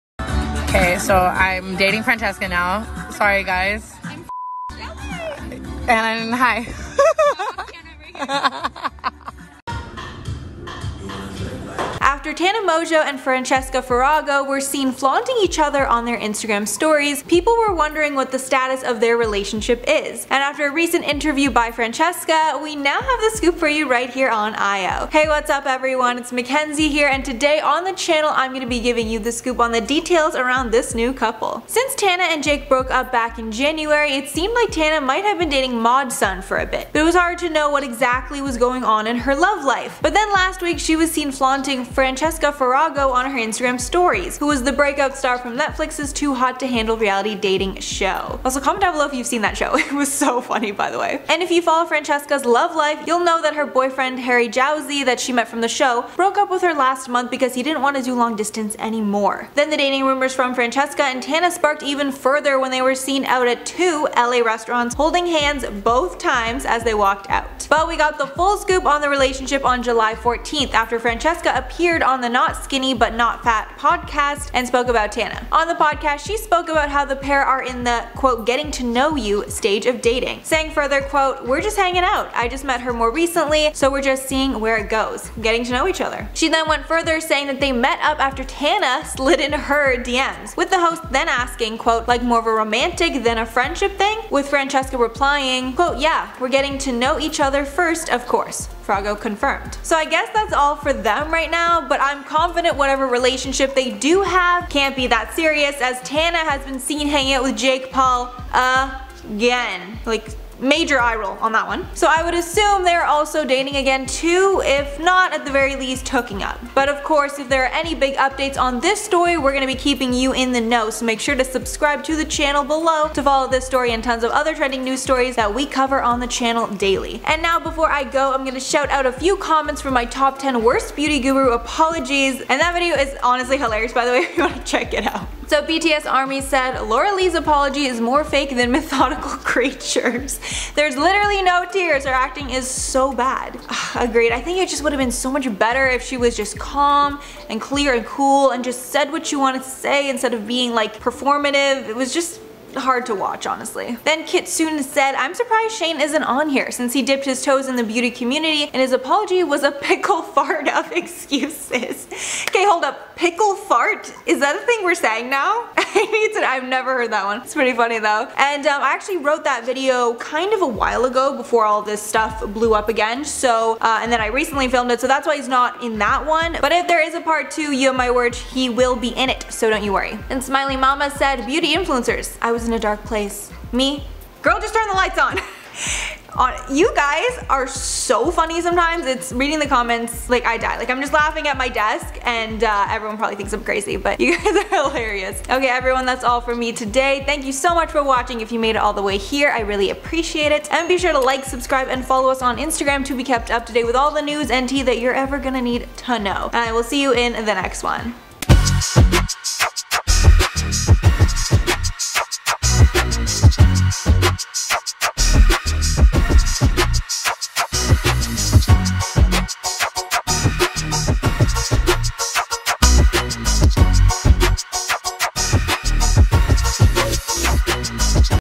Tana Mongeau And Francesca Farago Are DATING
After tana mongeau and francesca farago were seen flaunting each other on their instagram stories, people were wondering what the status of their relationship is, and after a recent interview by francesca, we now have the scoop for you right here on IO. hey whats up everyone its mackenzie and today on the channel im gonna be giving you the scoop on the details around this new couple/ Since tana and jake broke up back in january, it seemed like tana might have been dating modsun for a bit, but it was hard to know what exactly was going on in her love life. But then last week she was seen flaunting francesca farago on her instagram stories, who was the breakout star from netflix’s too hot too hande reality dating show.

FILMED BY:
Chris Stiuso